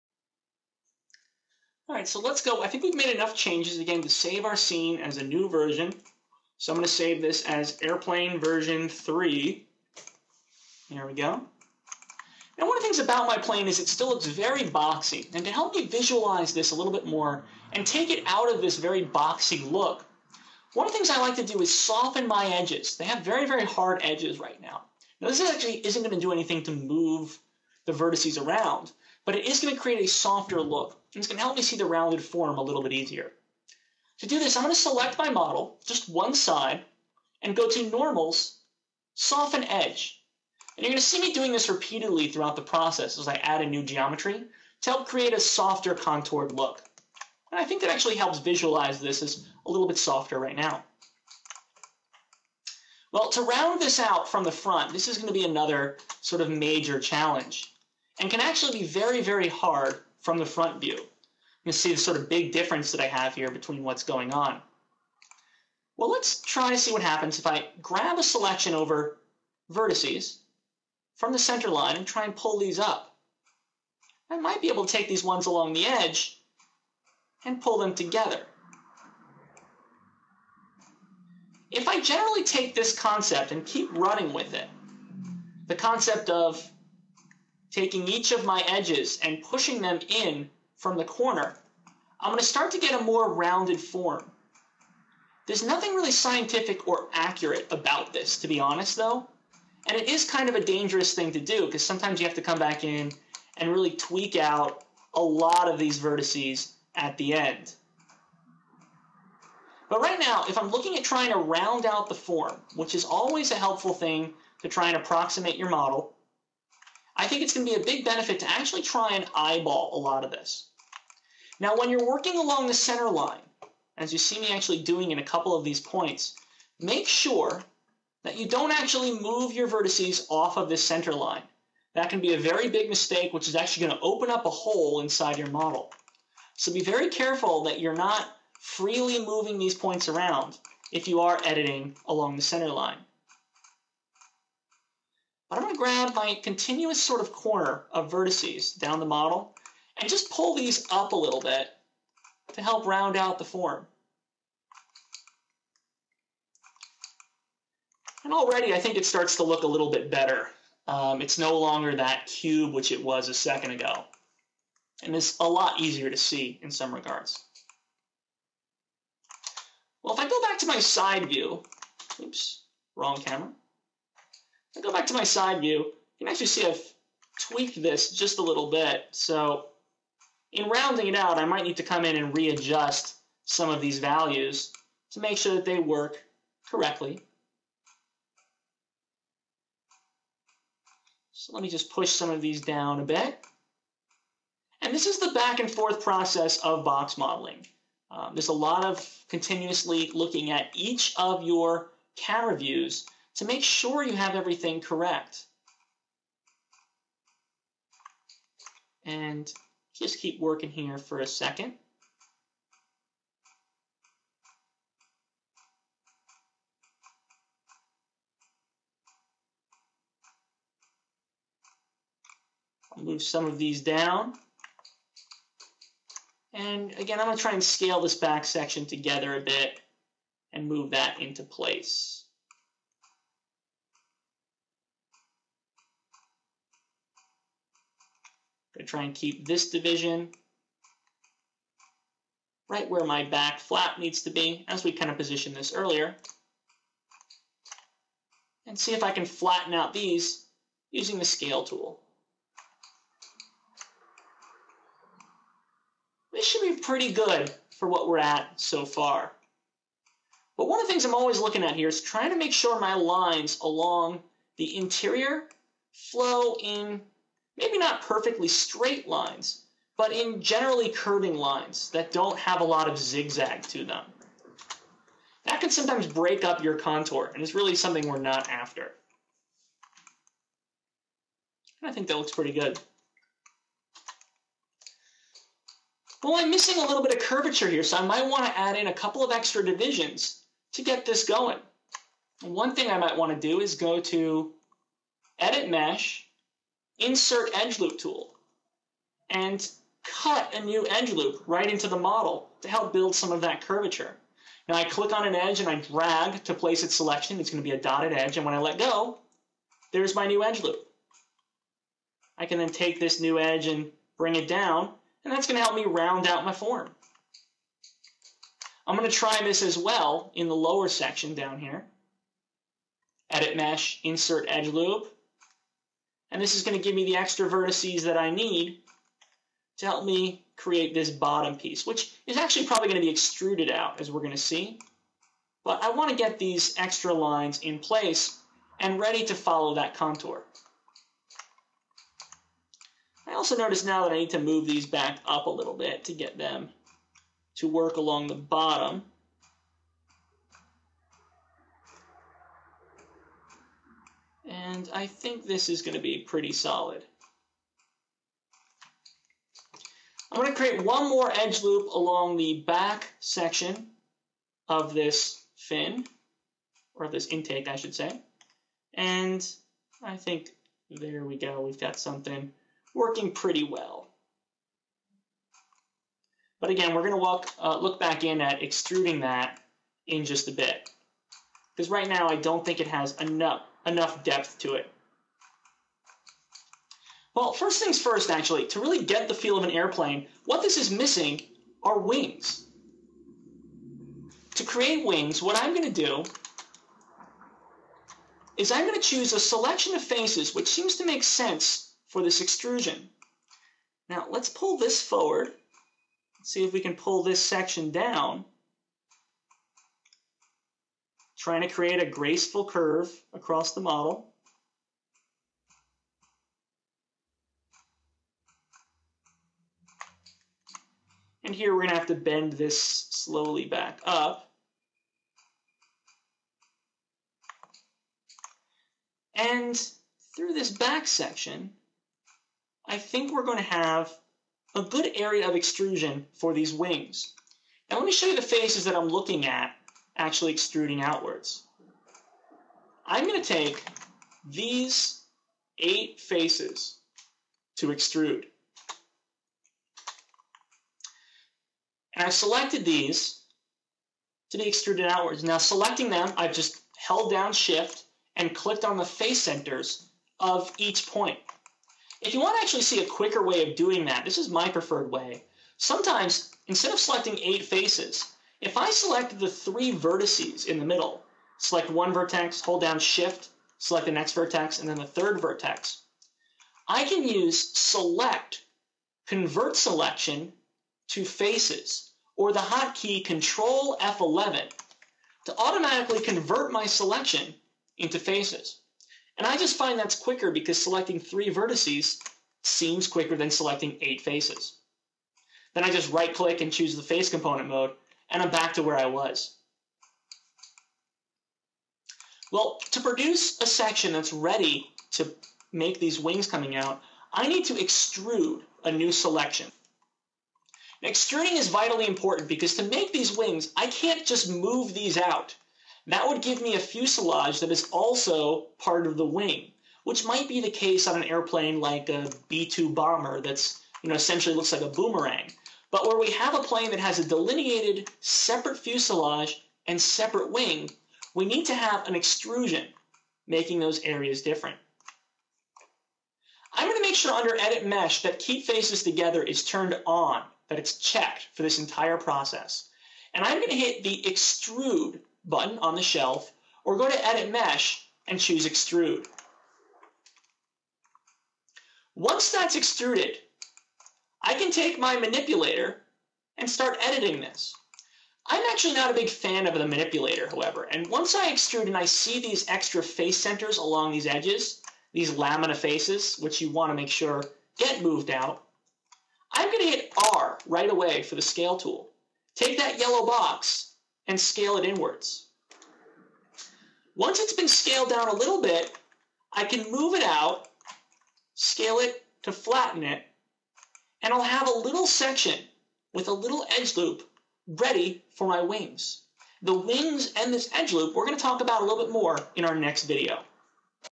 Maya Modeling Basics: Airplane pt. 6
In video 6 of 10, Andrew Klein rounds out the otherwise very boxy plane, and creates the first extrusion for the wings. This video introduces the extrude function and the insert edge loop function.